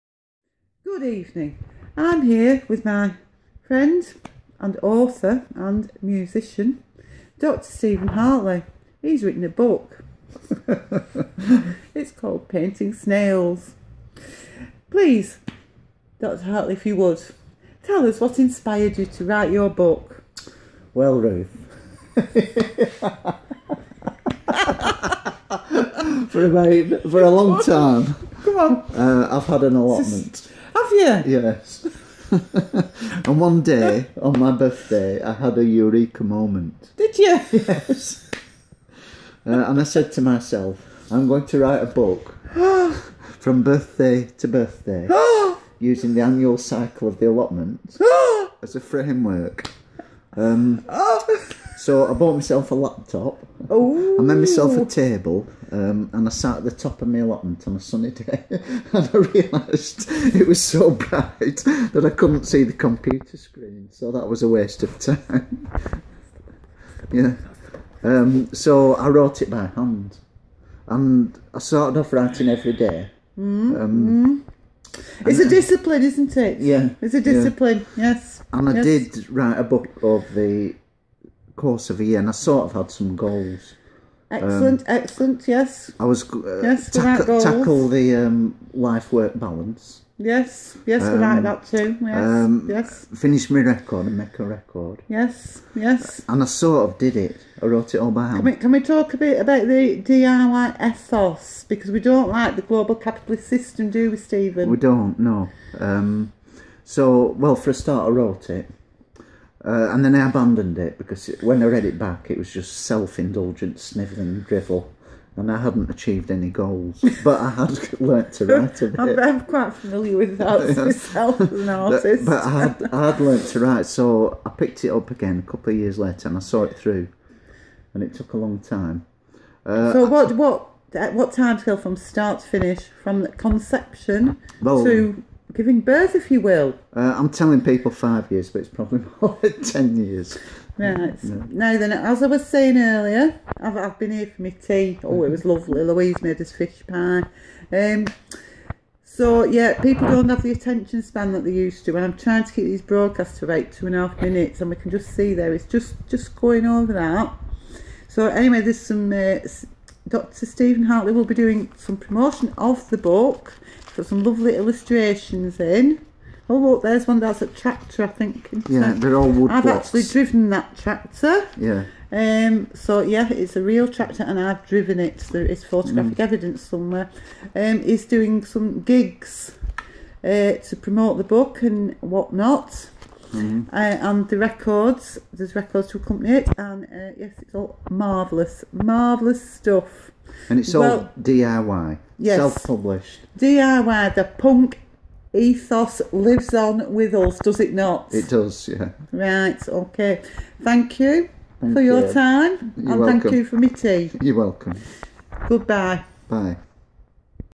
Painting Snails
An interview with renowned author Stephen John Hartley